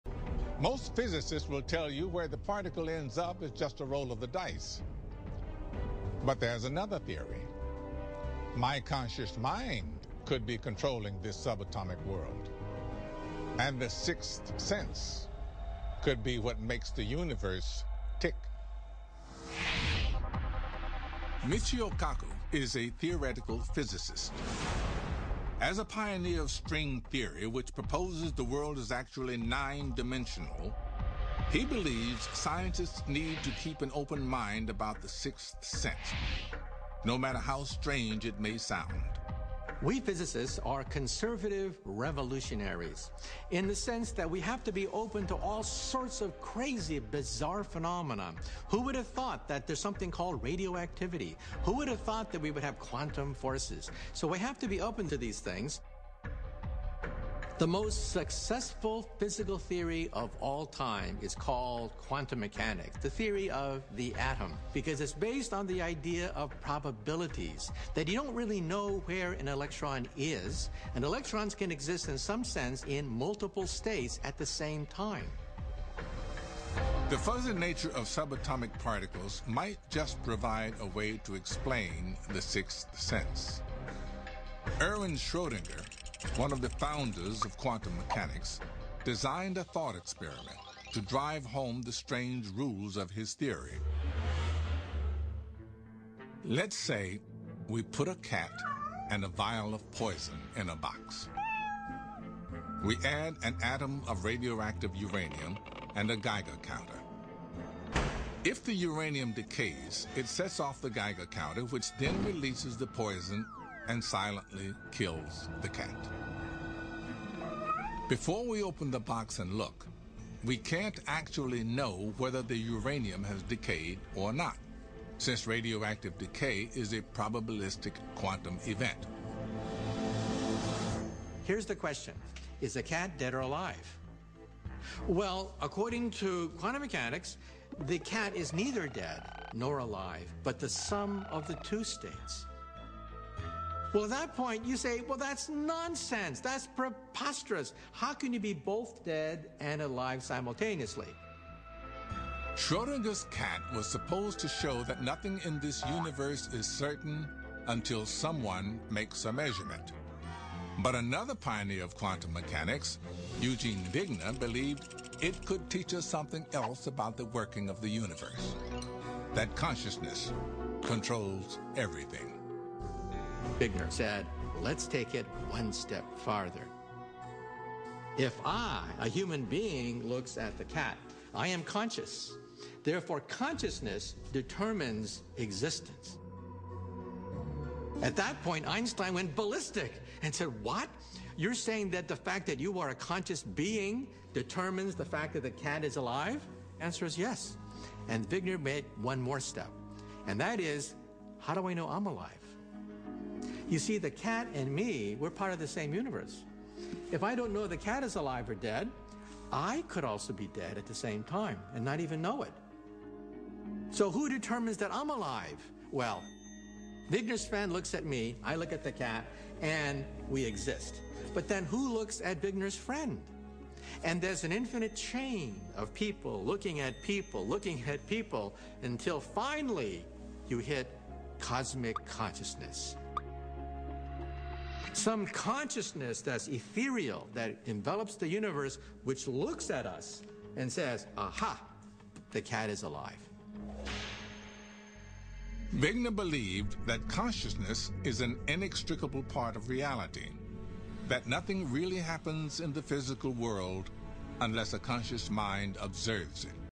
Quantum physics and consciousness
According to this theory, nothing really happens in physical world. Physical world is just a manifestation of the our consciousness. It seems that ancient Indian yogis have always known this. They have always believed in supreme consciousness which runs cosmos.